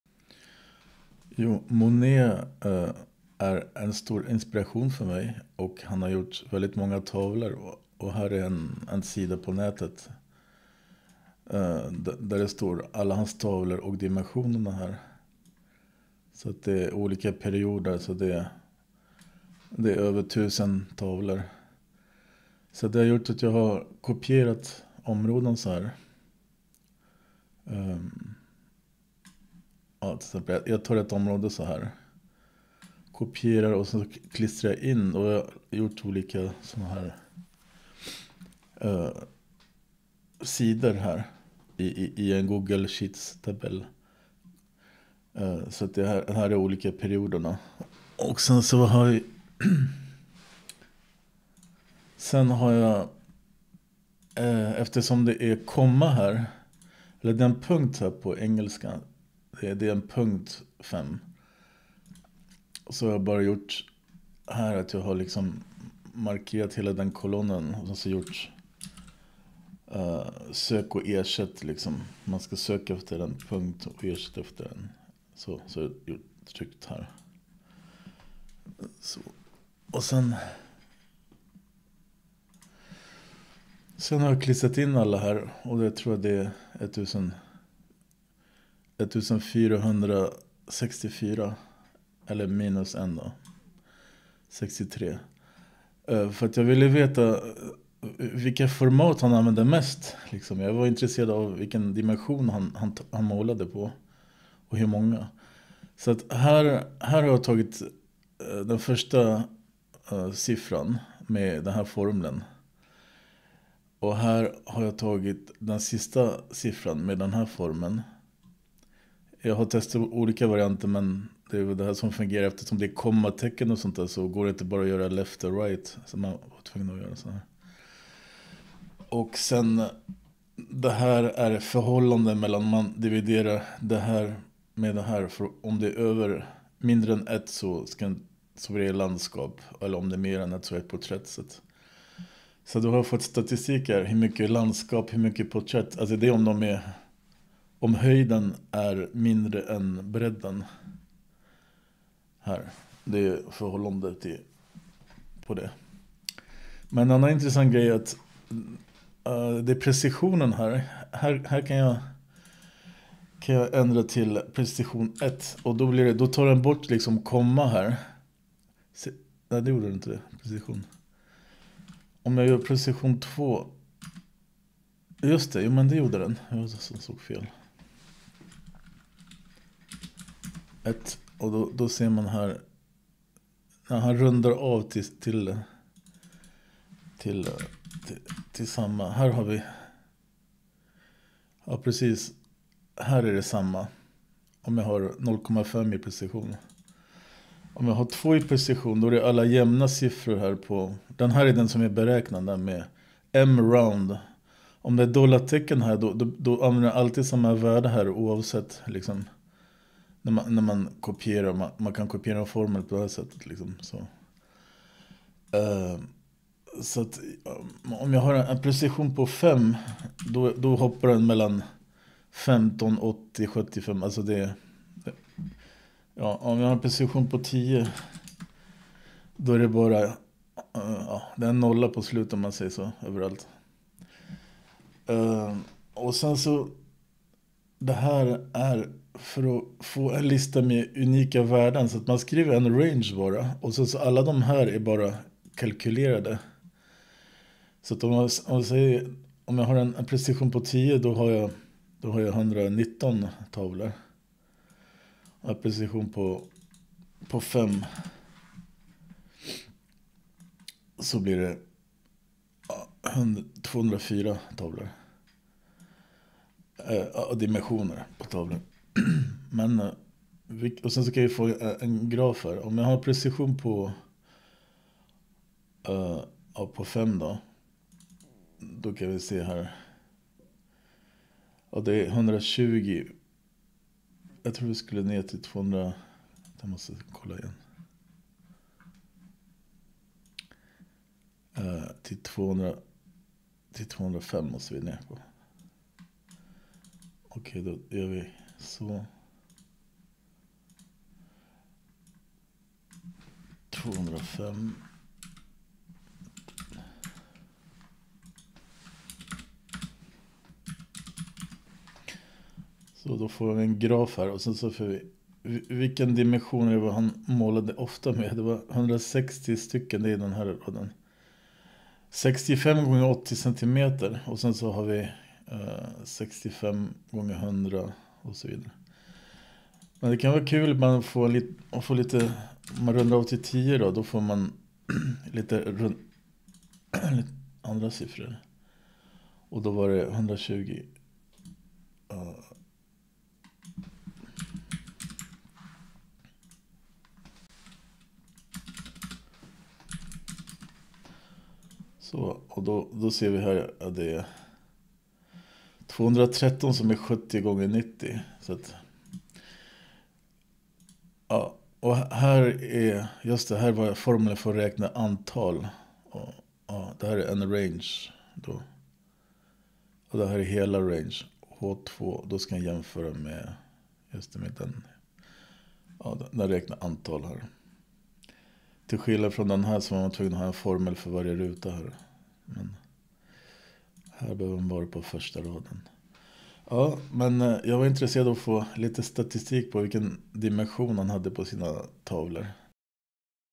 Tavelformat av Claude Monet
Här visar jag hur jag gjort för att få statistik på tavelformat av Claude Monet med Google sheets.

Chapters:
00:00 Lista på tavlor wikipedia
00:56 Sök och ersätt
01:48 Hämta höger och vänster värde
02:11 Format
02:39 Precision
04:00 Unika värden
04:33 Diagram